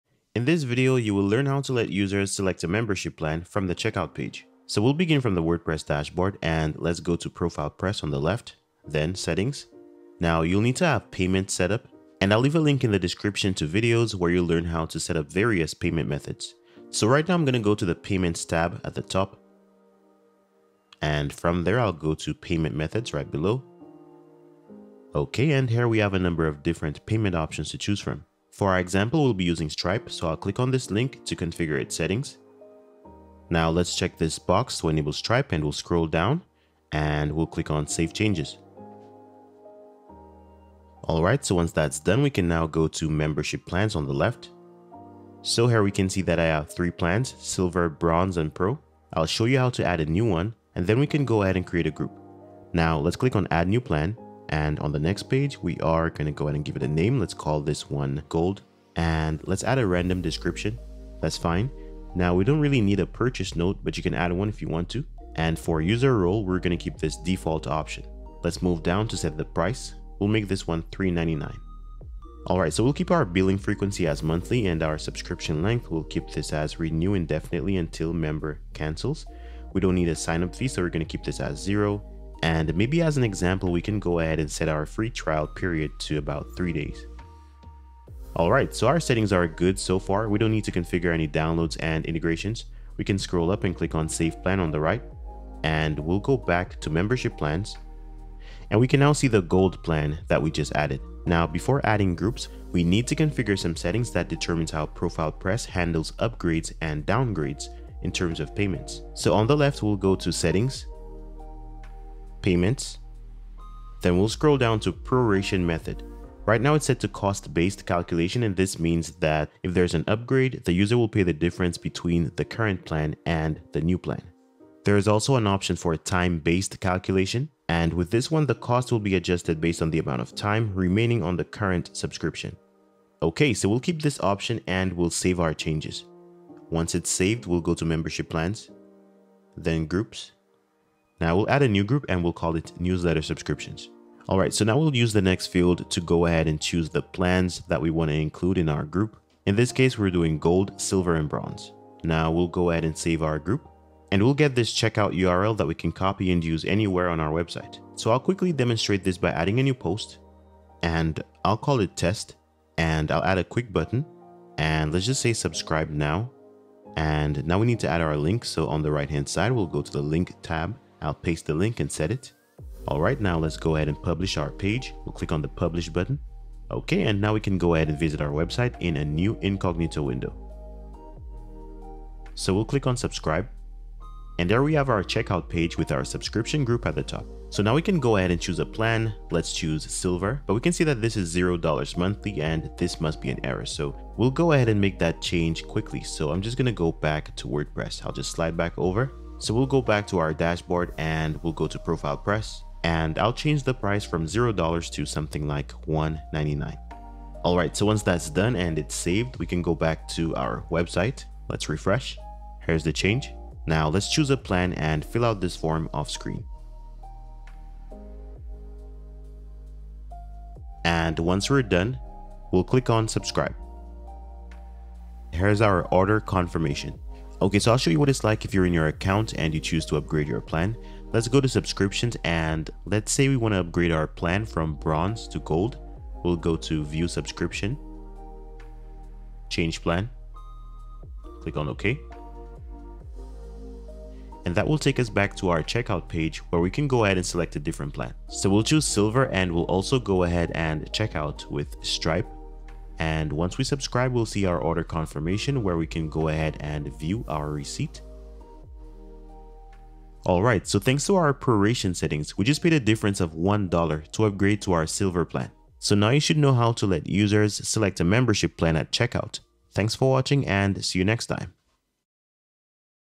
How to Let Users Select Membership Plans to Checkout in WordPress
As the world of online business continues to evolve, creating membership plans for your site and visitors is one of the most effective ways to monetize your website. However, enabling users to select the correct membership plan during checkout can be daunting.

Fortunately, with the ProfilePress WordPress membership plugin, you can easily create multiple membership plans, integrate various payment methods, and allow group checkout on your WordPress site.

By the end of this video, you will have all the knowledge you need to create an entire membership site with WordPress that maximizes your revenue potential.